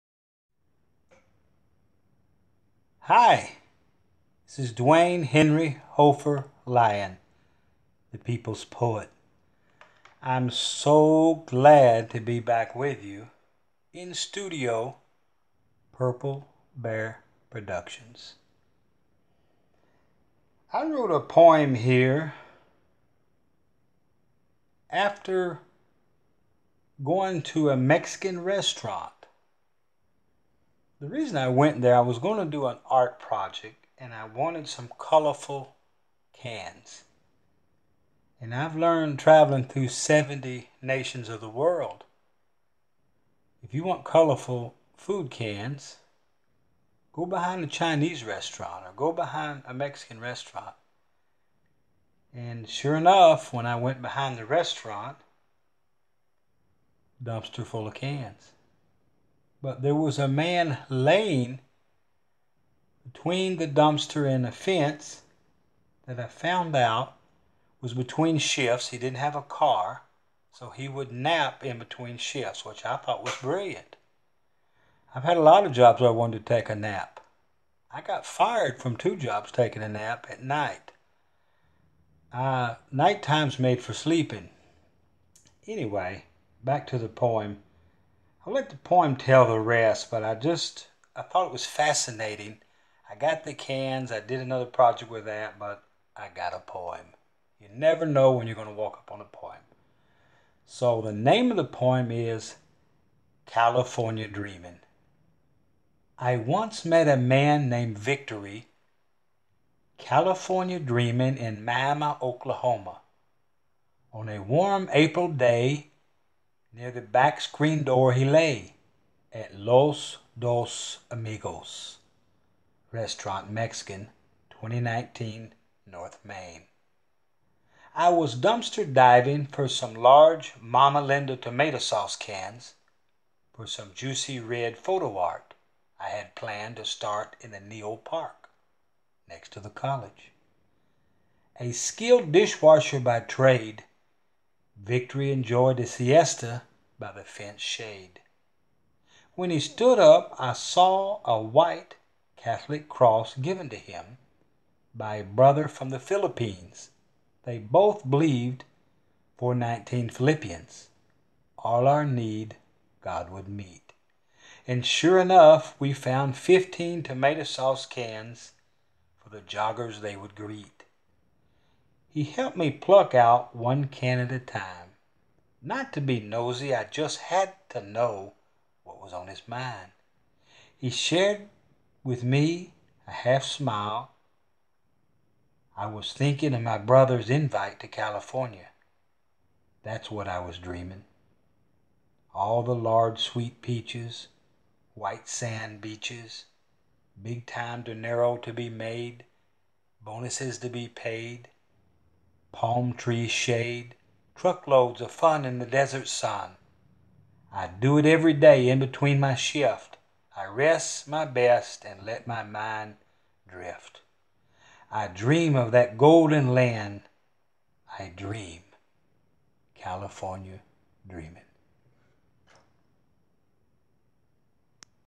"CALIFORNIA DREAMING "Duane Henry Hoefer Lyon-poet
True story! Miami Oklahoma, the restaurant is still in operation, I don't know if my brother made it to California yet or not. he is a delightful man who inspired me with his simplicity, some one once asked me how do you obtain all the material for your poetry? The people I meet daily provide me everything I need to, hence I was once titled by a international newspaper the people's poet. I have studied people all around the world, well in 70 countries to date! I love people, I love you! Duane Henry Hoefer Lyon, the people's poet! streetpreacher .Purple Bear Productions, Jordan Kentucky, USA and Manitoba Canada, . ...thank you kindly friends!